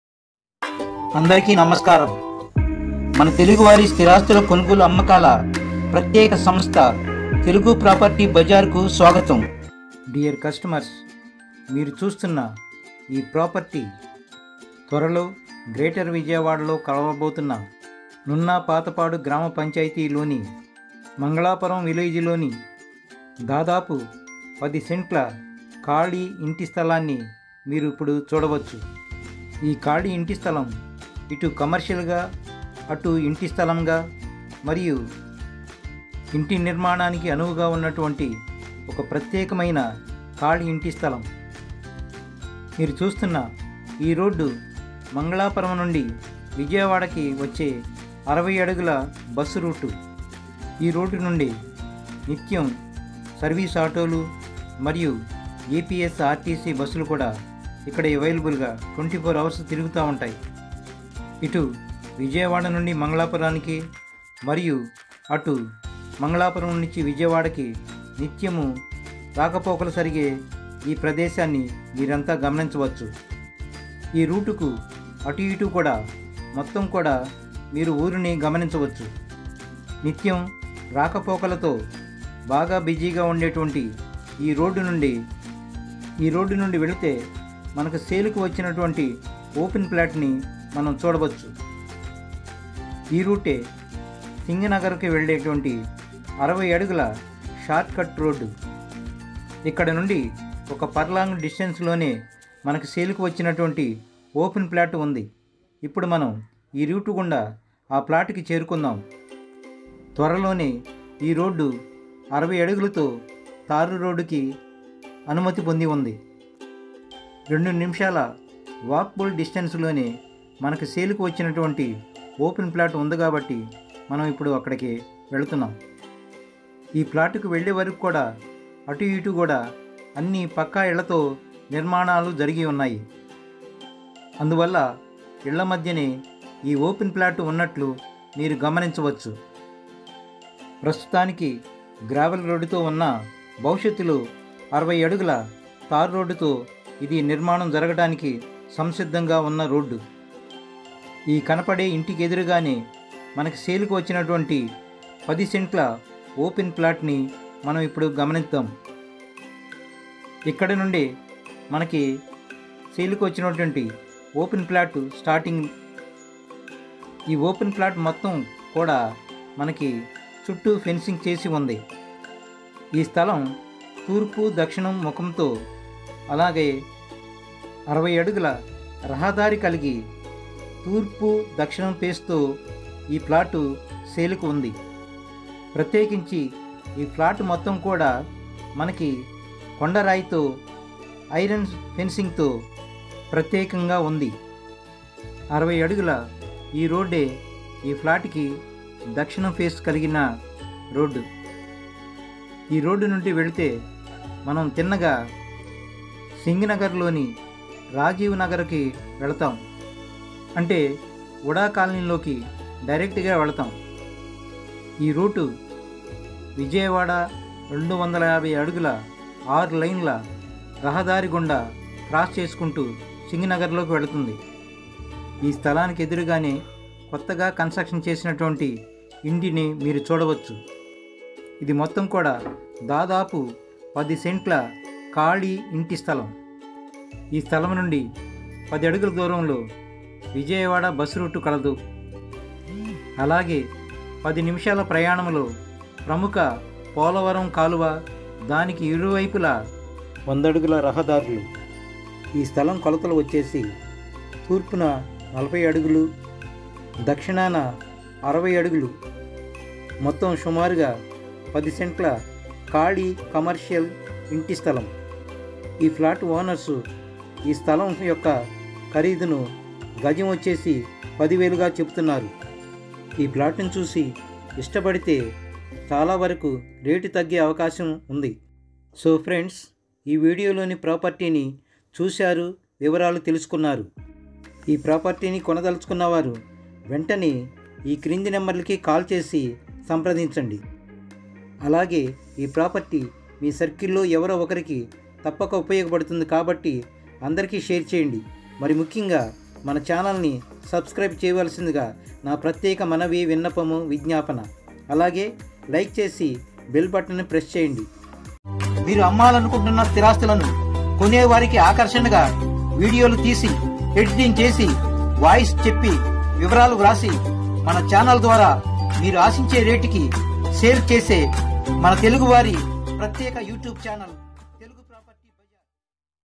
VIJAYAWADA 250 sft బై పాస్ దగ్గరలో east+south corner 60 sft రోడ్డు ఆనుకోని10 cents open plot
1 . AREA VIJAYAWADA NUNNA PATHAPADU GRAMA PANCHAYT MANGALAPURAM VILLAGE.
2 . 60FT GRAVEL ROAD .
3 . EAST + NORTH CORNER BIT .
4 . TOATAL LAND AREA = 10 CENTS OPEN LAND .
5 . 10 FTS  DISATANCE TO  BUS STOP .
6 .  BELOW 1/2 KM " 250FT  6 LINES VIJAYAWADA BYPASS ROAD .
7 .  BELOW 1KM " SINGHNAGAR .
8 . 1/2 KM DISTANCE TO POLAVARAM 100 FT ROAD.
9 . PRICE : 10,000 PER SQ YARD .